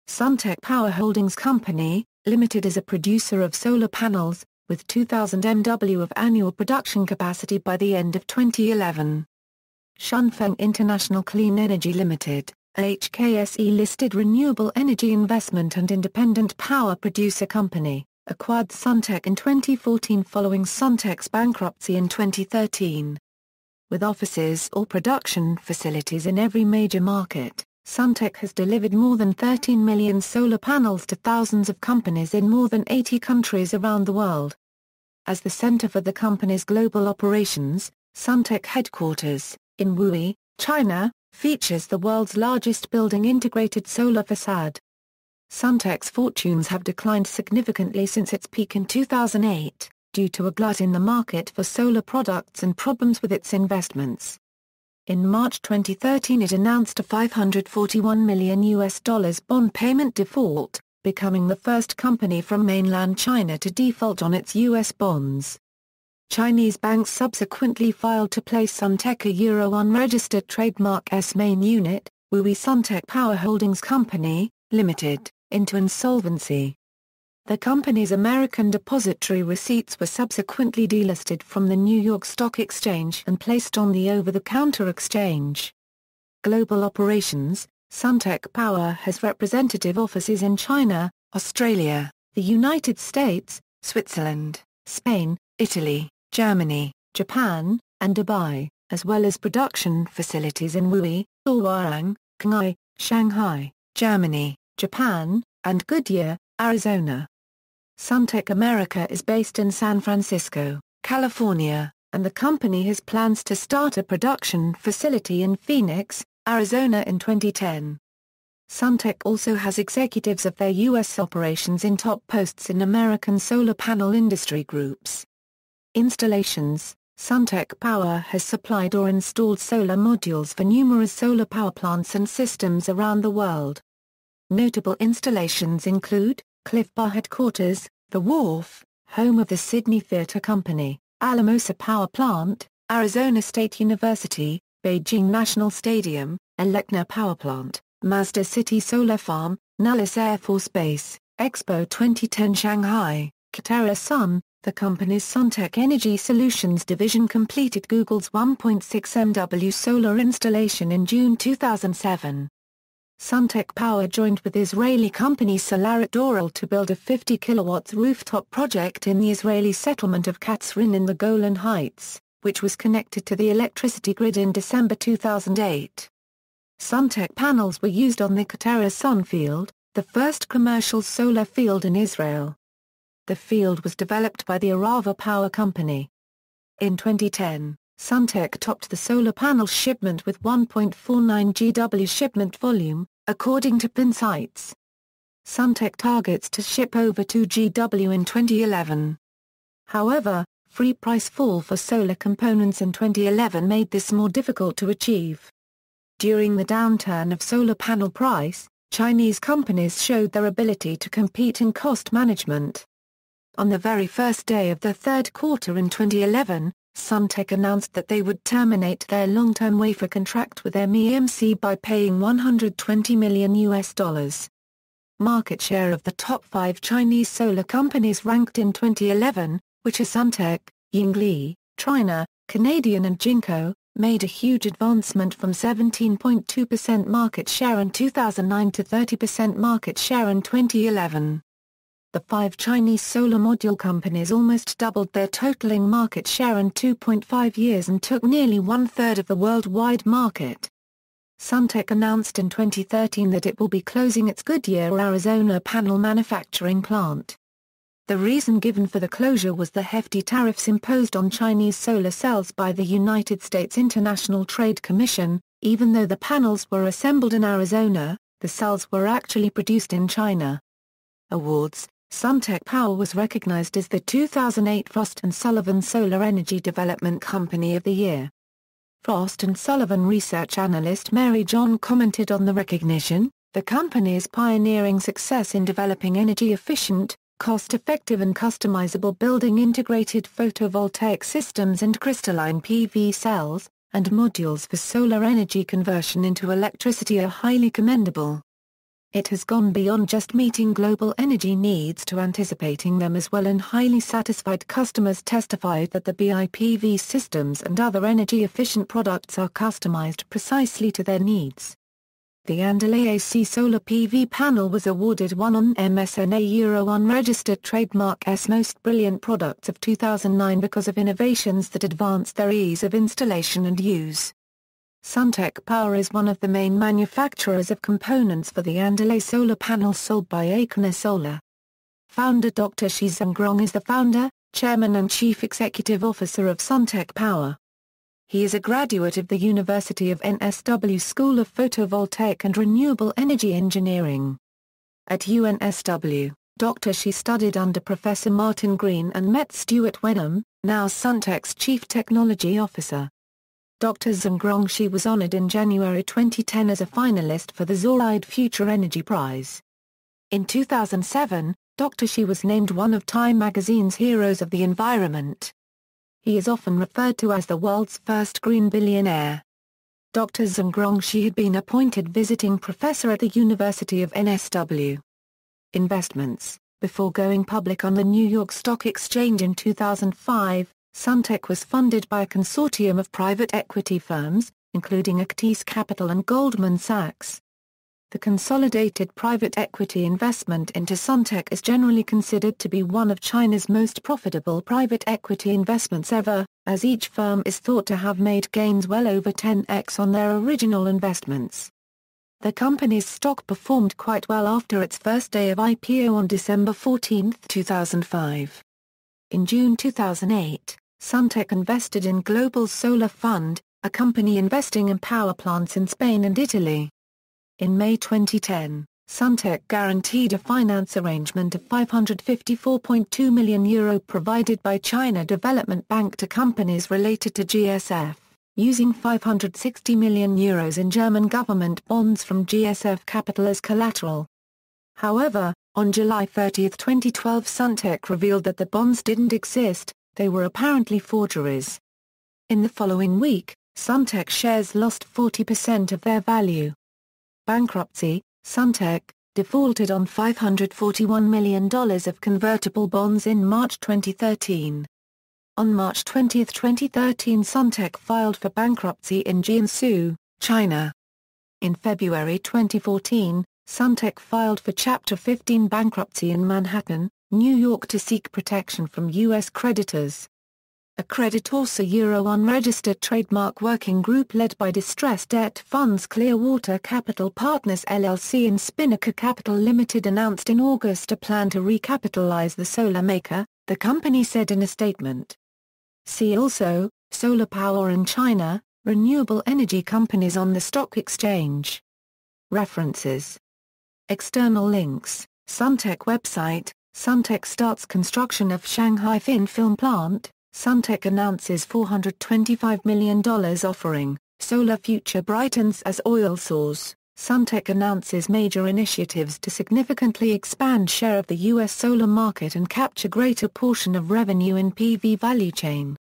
Suntech Power
Suntech Power Holdings Co., Ltd. is a producer of solar panels, with 2,000MW of annual production capacity by the end of 2011. Shunfeng International Clean Energy Limited, a HKSE listed renewable energy investment and Independent Power Producer company, acquired Suntech in 2014 following Suntech's bankruptcy in 2013. With offices or production facilities in every major market, Suntech has delivered more than 13,000,000 solar panels to thousands of companies in more than 80 countries around the world. As the center for the company's global operations, Suntech headquarters, in Wuxi, China, features the world's largest building integrated solar facade.
Suntech's fortunes have declined significantly since its peak in 2008, due to a glut in the market for solar products and problems with its investments. In March 2013 it announced a US$541 million bond payment default, becoming the first company from mainland China to default on its US bonds. Chinese banks subsequently filed to place Suntech’s main unit, Wuxi Suntech Power Holdings Co., Ltd., into insolvency. The company's American Depository Receipts were subsequently delisted from the New York Stock Exchange and placed on the over the counter exchange.